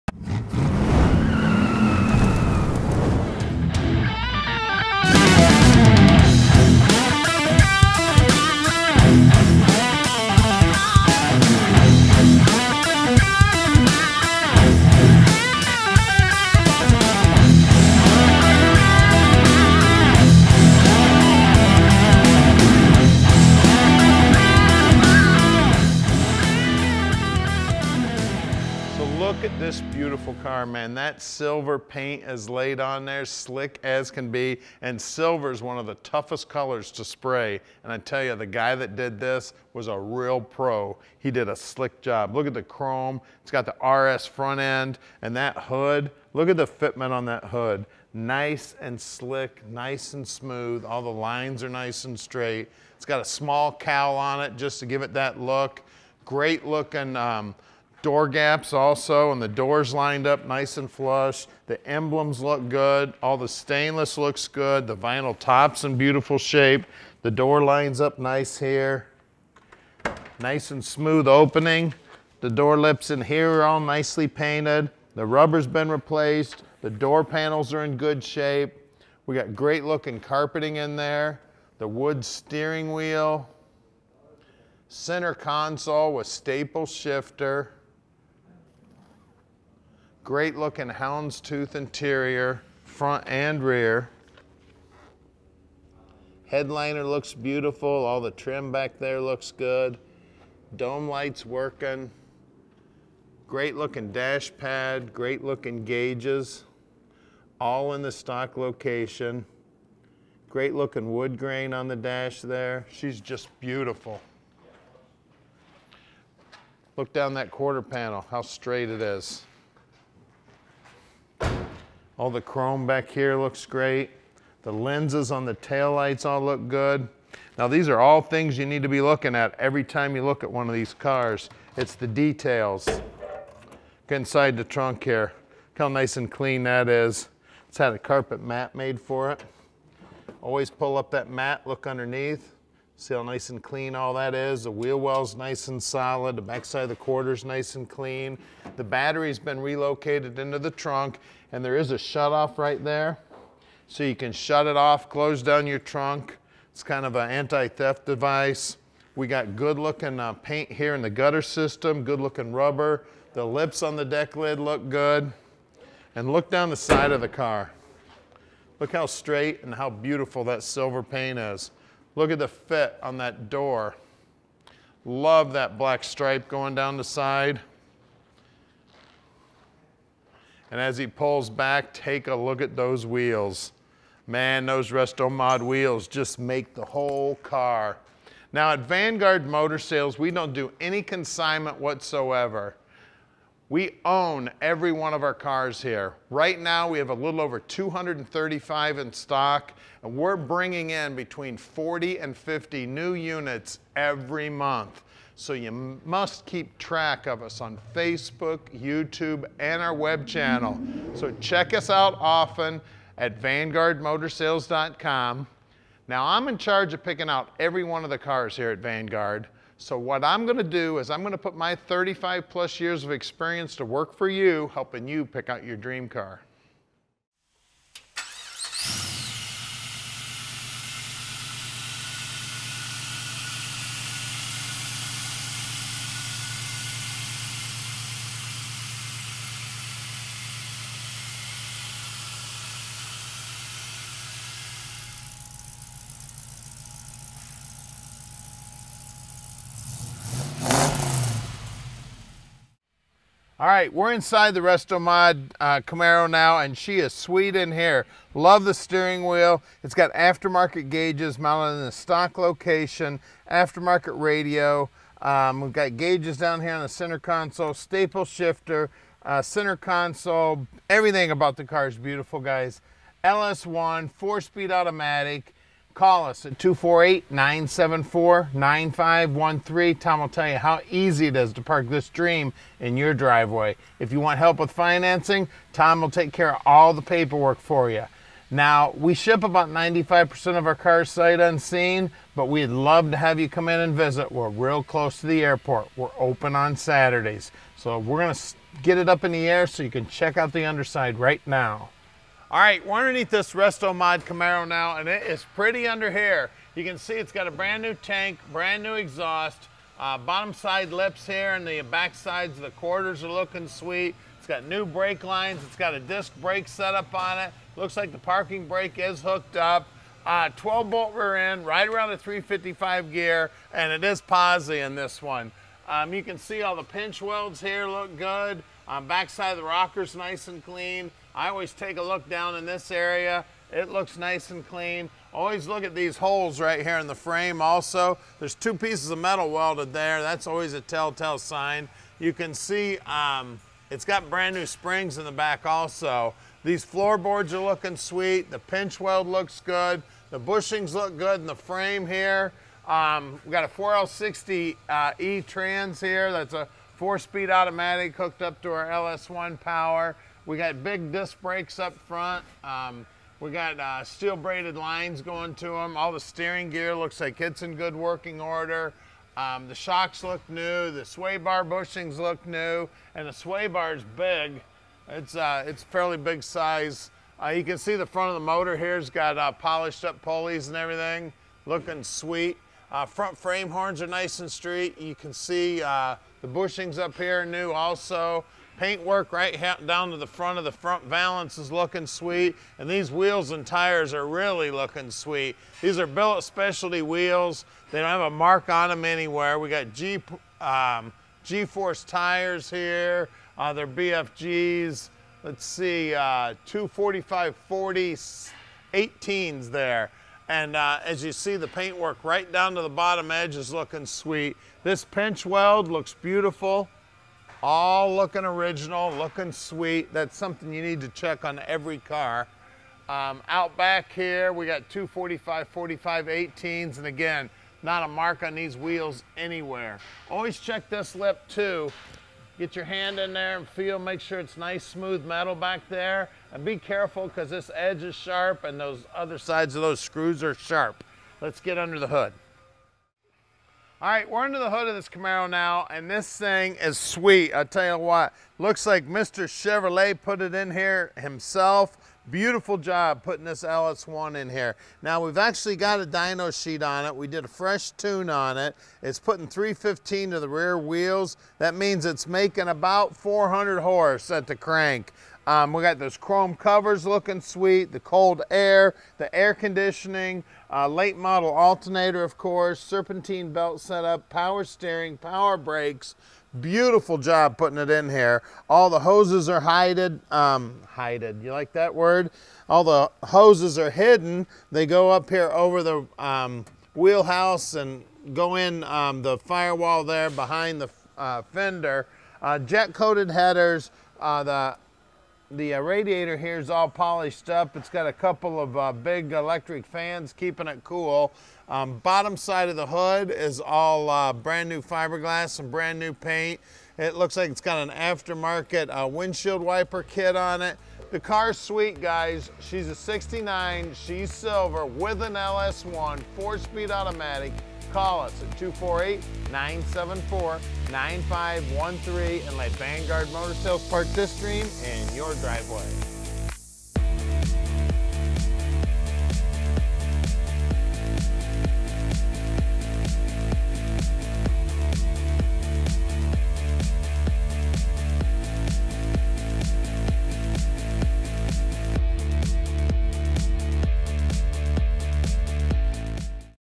1969 Chevrolet Camaro For Sale
1969 Chevrolet Camaro Restomod

VIN: 124379N621025

This Camaro has undergone a full rotisserie restoration. Under the hood sits an LS1 V8 connected to a 4L60E automatic transmission with stage 2 upgrades pushing all the power to the rear axle. Routing exhaust gases to the exhaust is a set of Doug’s headers. Adding to the driving experience is Power Steering, Power 4-Wheel Disc Brakes, and Ice-Cold A/C. The dash has been upgraded with Dakota VHX gauges. In front of those new gauges, there is a tilt column with a rosewood steering wheel. You can bet that you will be able to listen to your music in this beauty with its new Retro Sound Radio. Drive anywhere in this beautiful Camaro!

to see over 100 HD photos and our in-depth video for this 1969 Chevrolet Camaro and over 200 other classics!

Mechanics:
GM LS1 V8
Engine # 12561158
Behind Drivers Cyl Head
Stock Heads
92/93 Octane
Electronic Fuel Injection
4L60E 4 Speed Automatic Transmission
Trans # 24206952
Front Suspension: Independent
Rear Suspension: Leaf Spring
12 Bolt
- Ratio: 3.55:1
- Posi
Brakes:
Front - Power Disc
Rear - Power Disc
Exhaust:
Dual Exhaust
Stainless Steel

Underneath:
- Gas Tank is New
- Straps are New
- Spring Packs Rebuilt
- New Brake Lines
- U Bolts in Good Shape
- After Market Rear End
- Exhaust looks New
- New Fuel Lines
- Floor Boards look Great
- Frame is Solid
- Back of Rockers look Good
- Pinch Welds are Nice
- Bushing in Great Shape
- Parking Brake Cable looks Good
- Headers in Good Shape
- Control Arms in Good Condition
- Springs are New
- Sway Bar looks New

Body:
- RS Front End
- Gorgeous Silver Paint
- Hood Lines Up Well
- Doors are Flush
- Rocker Moldings look good
- Stainless in Good Shape
- Vinyl Tops is Beautiful
- Door Handle looks New
- Door Jams Painted nicely
- Rear Chrome Bumper looks Good
- Black Out Tail Panel
- Trunk Opens Nicely
- Wheel Wells are Tight
- Carpet in Trunk in good Shape
- Paint in Gutters looks Good
- Bottom of Deck Lid looks Great
- Rubber Replaced
- Lips on Deck Lid in Great Shape

Interior:
- Rubber in Great Condition
- Deluxe Door Panel looks Great
- Simulated Wood Steering Wheel
- Center Console in Good Shape
- After Market Radio
- Hounds Tooth Interior
- Front and Rear Seat Belts
- Head Liner in Good Condition
- Trim in Great Shape
- Dash Pad in Good Shape
- Gauges look Amazing
- Stock Located
- After Market Gauges